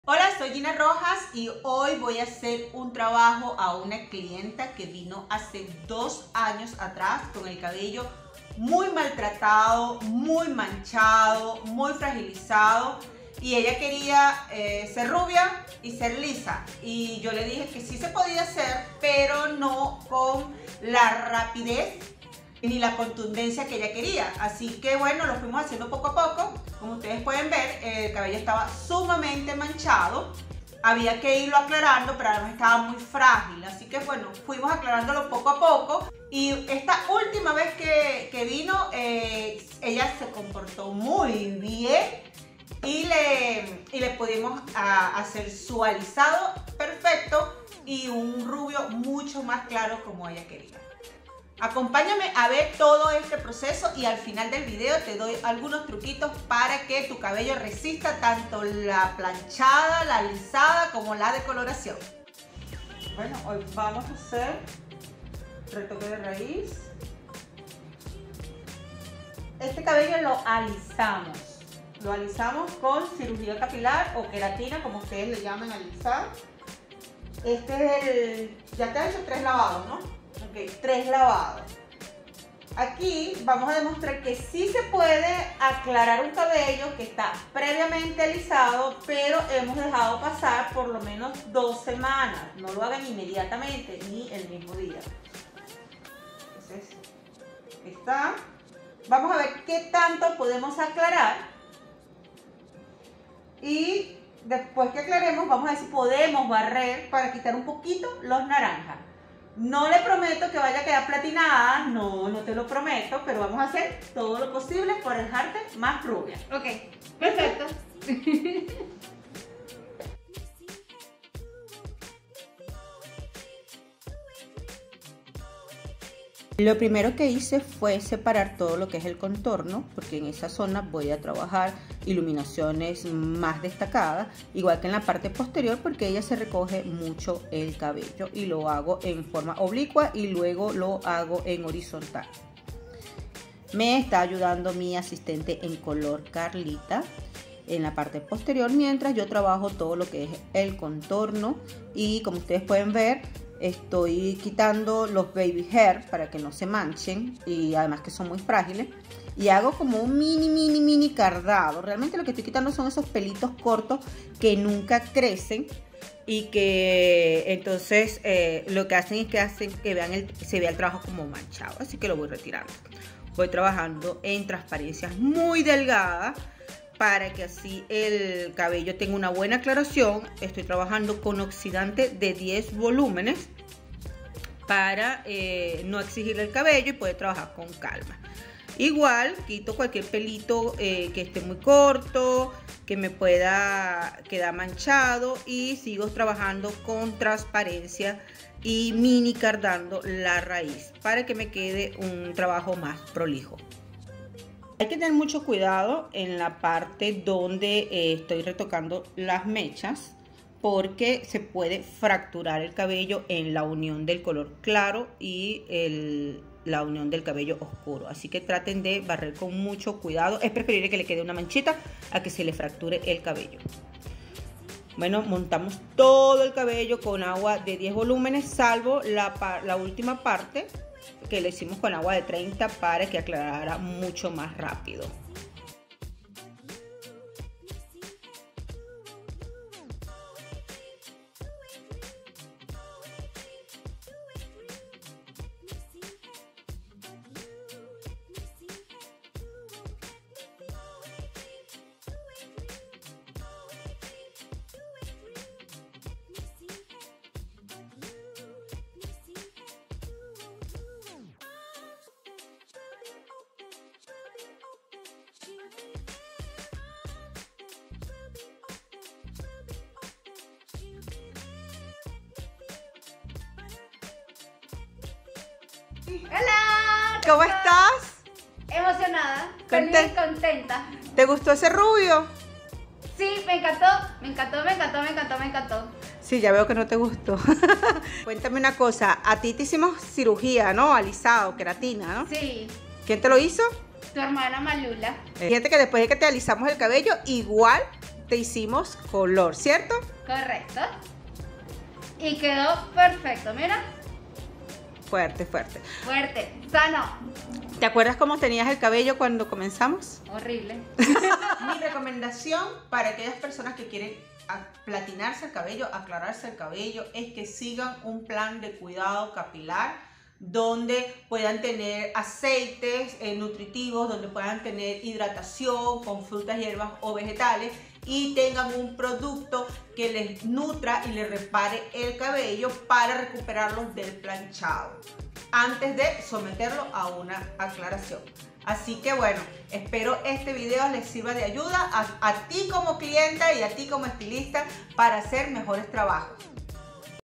Lisa y Rubia vs Cabello Manchado y Maltratado
Mi clienta necesitaba un retoque de raíz además de que requería corrección de color, en este video te voy a explicar cómo eliminar manchas en un cabello previamente alisado.

Gina Rojas - Estilista - Colorista – Maquilladora - Venezolana 💜